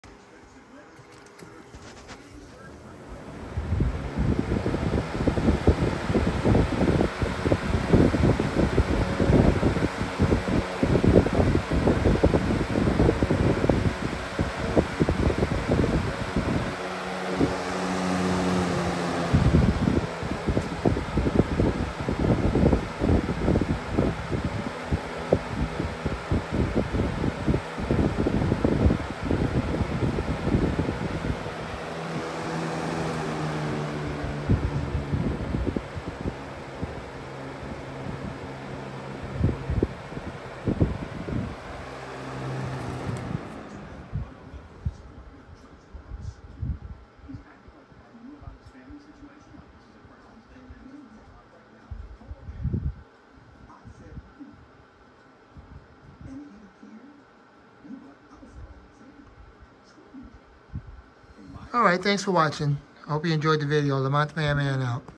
Lasko B20308 (discontinued Royal Blue Color)Box Fan
New blade replacement original blade sat and its unfortunately out of balanced but moves the air well